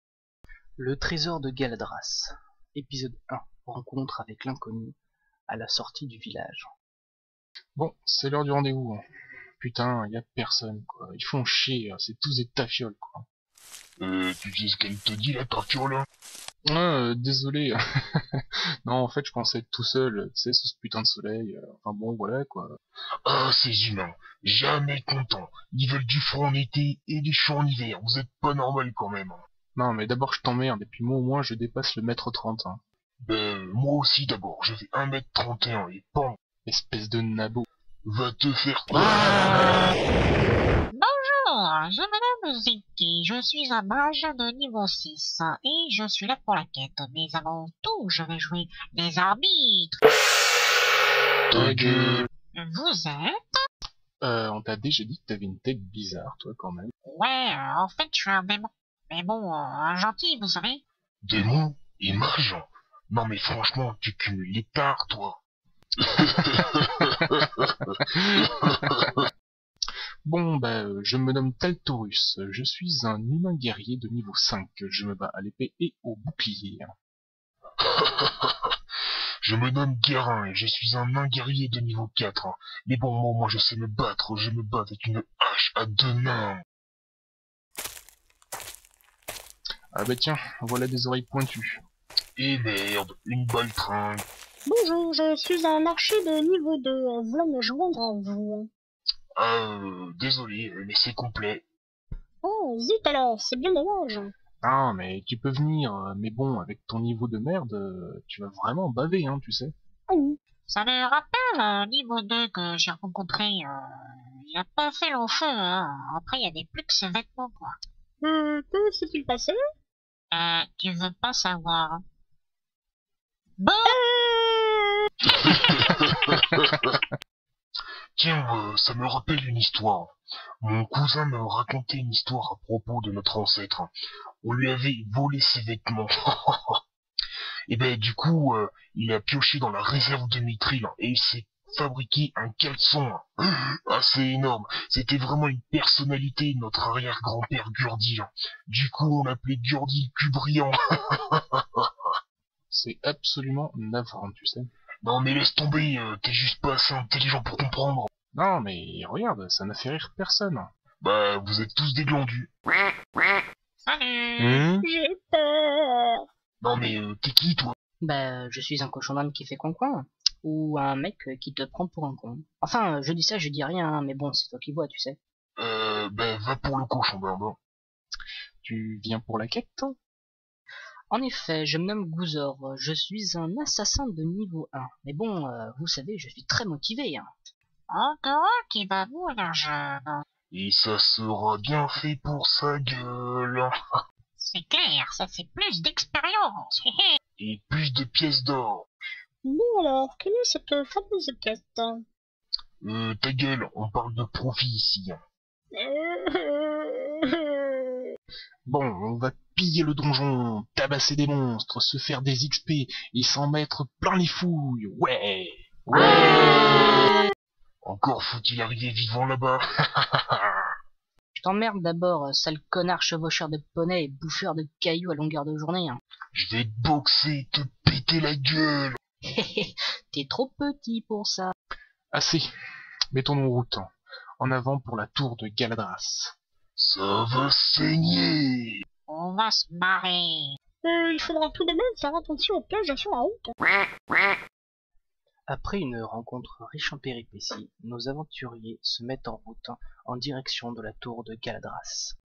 Le trésor de Galadras - Episode 1
Voici une équipe d'aventurier qui décide de partir à l'aventure en quête de renom, d'expérience et de richesse.